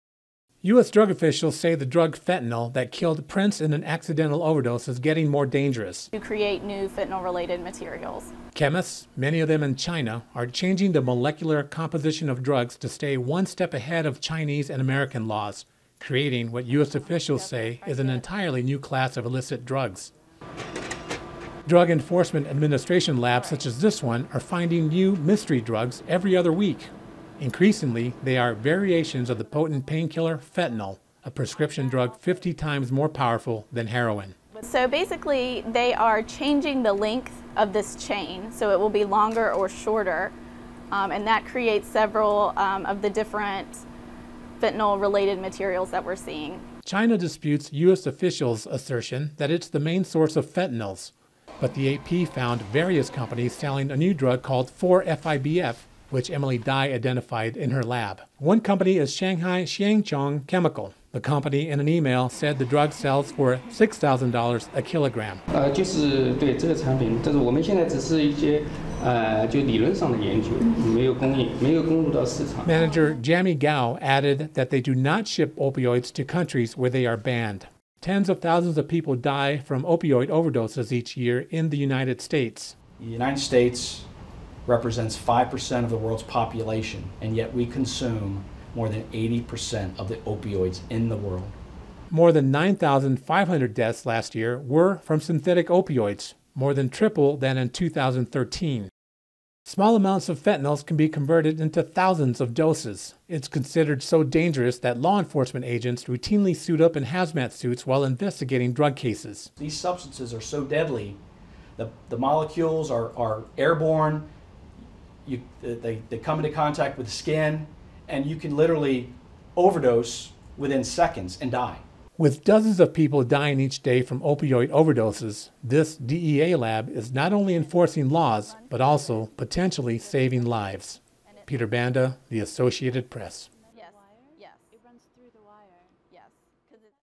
Chemists Create New Class of Drugs to Avoid Law
The proliferation of a rapidly evolving synthetic opioids has become so fierce that the DEA says they now constitute an entire new class of illicit drugs, built to mimic the powerful painkiller fentanyl. (Dec. 21)